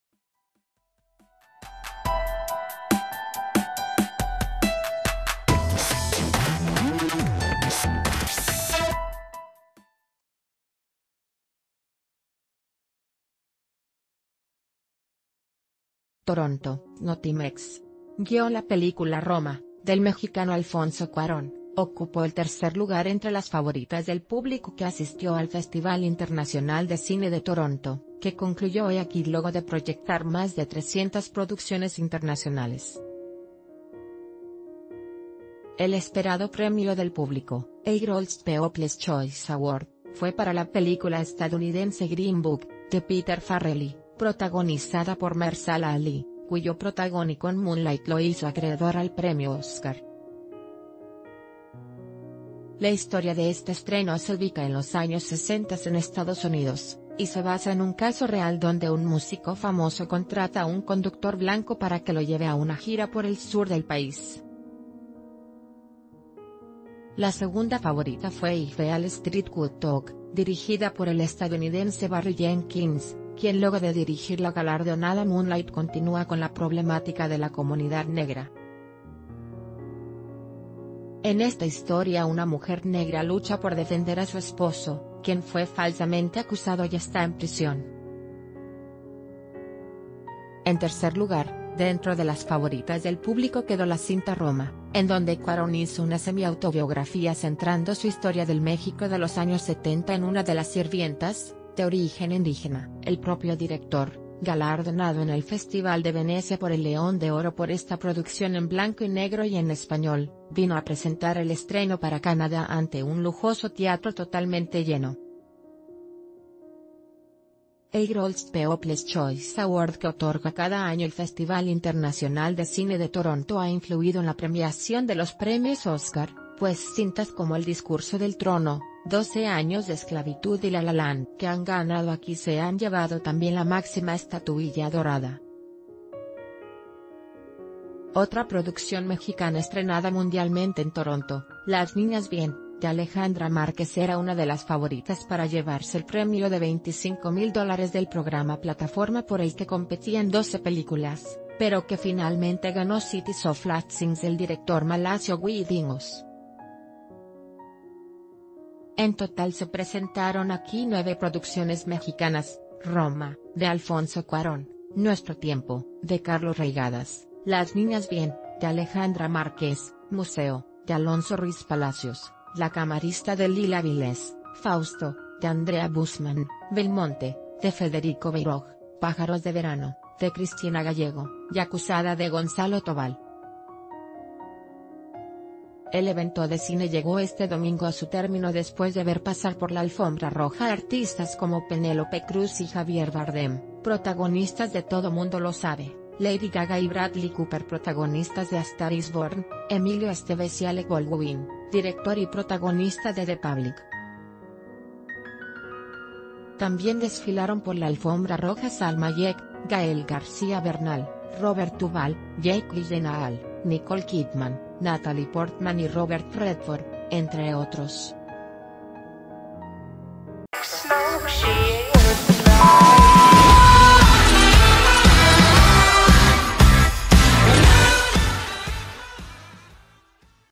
“Roma“, de Alfonso Cuarón, tercer lugar en cine de Toronto - El Diario de Yucatán
Toronto (Notimex).- La película “Roma”, del mexicano Alfonso Cuarón, ocupó el tercer lugar entre las favoritas del público que asistió al Festival Internacional de Cine de Toronto, que concluyó hoy aquí luego de proyectar más de 300 producciones internacionales. El esperado premio del público, el Gr...